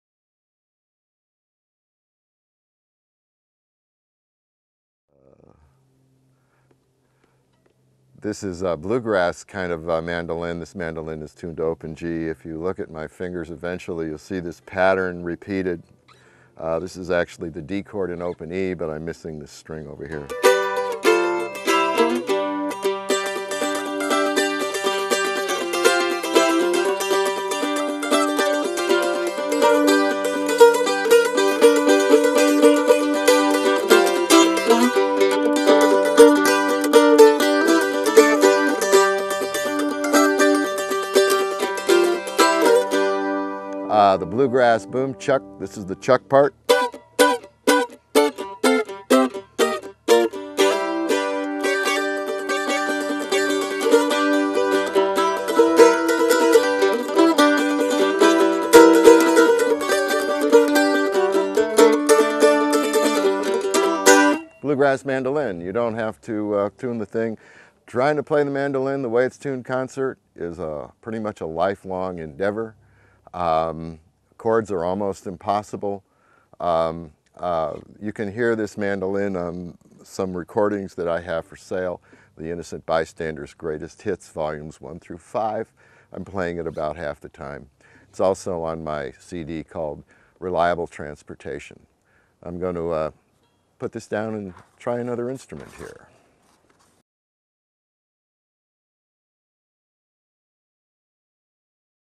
Open Tuning Part 3-Mandolin
The first in the series of many clips covering open tuning and it's many advantages as well as it's unique history and implementation in modern day music culture. Performed and produced by Ed Dowling(musician, technophonic luthier, and mechanical engineer,)Que' Lastima Productions, and directed by Suki Dejong, uploaded by me..